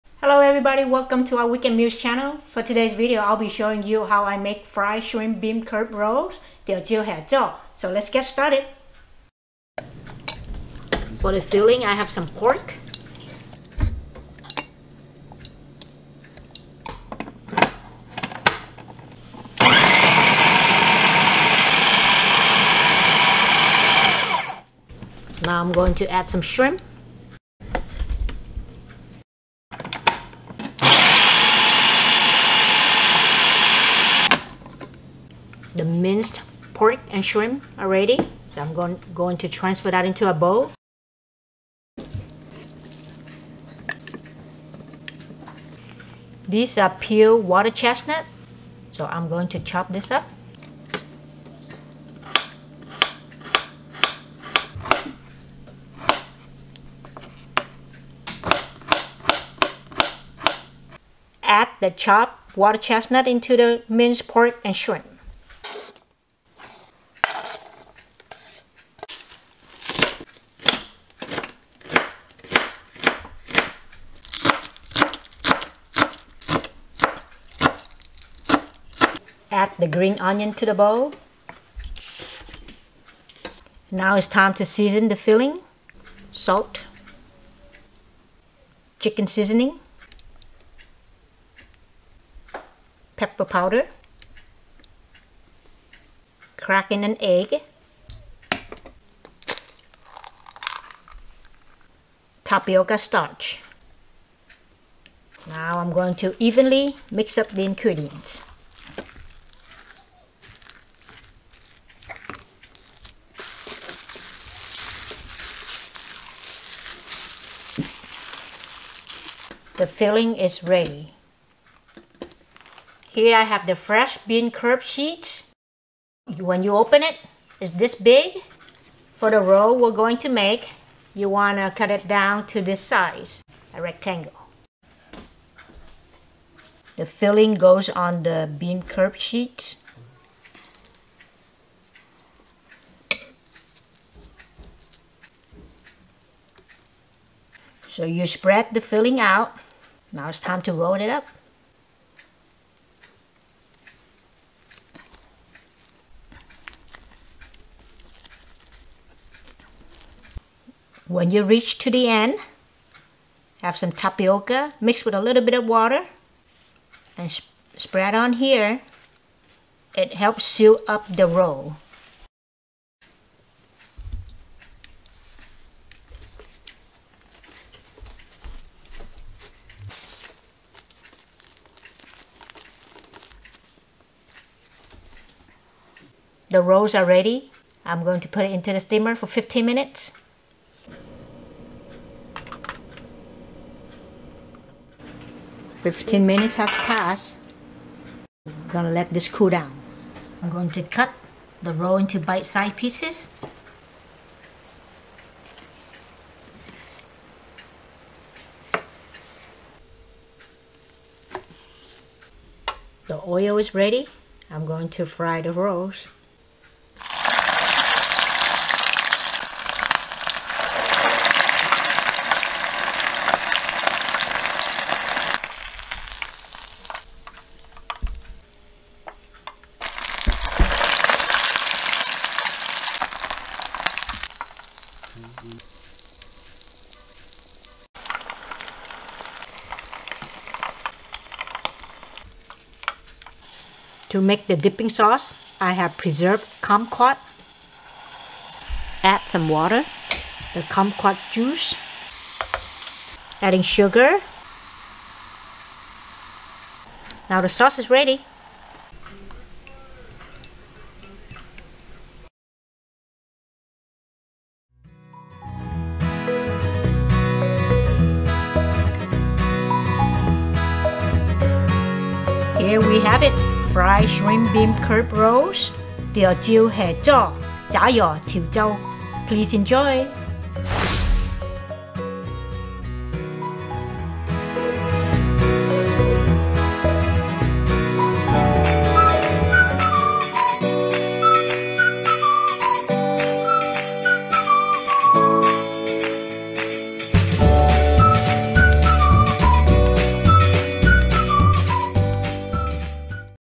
Fried Shrimp Bean Curd Rolls (Chả Giò Triều Châu), 潮州蝦棗 [CC Added]
Welcome to our Weekend Meals channel. Today's Weekend Meals will show you how to make Fried Shrimp Bean Curd Rolls.  A similar version to Vietnamese egg rolls but this recipe uses bean curd which brings out a fabulous, crunchy, and delicious appetizer that everyone can enjoy.  I hope you like the Fried Shrimp Bean Curd Rolls recipe. Thanks! 💗💗💗

📑 Ingredients:
1 lb Minced Pork
1 lb Minced Prawn
8 oz Minced Water Chestnut
2 Stalks Spring Onion (chopped)
1 Egg
1 tsp Salt
1/4 tsp Powder Pepper
1 tsp Chicken Seasoning
1 tbsp Tapioca Starch
8 oz Bean Curd Sheets

Dipping Sauce:
Preserved Kumquat
Water
Sugar